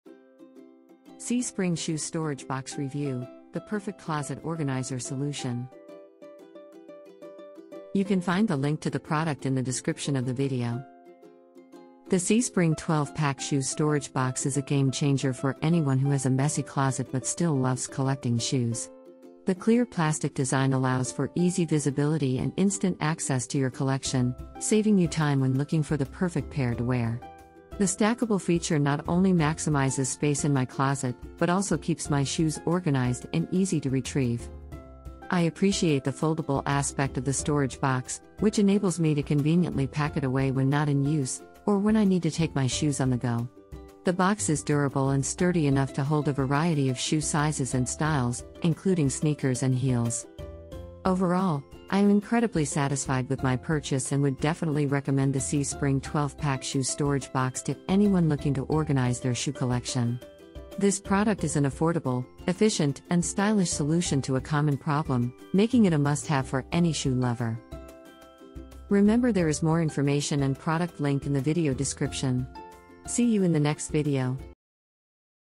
SEE SPRING Shoe Storage Box Review: The Perfect Closet Organizer Solution!
SEE SPRING 12 Pack Shoe Storage Box, Clear Plastic Stackable Shoe Organizer for Closet, Space Saving Foldable Shoe Sneaker Containers Bins Holders

00:00 - Intro
00:10 - 1) SEE SPRING 12 Pack Shoe Storage Box, Clear...
01:07 - Outro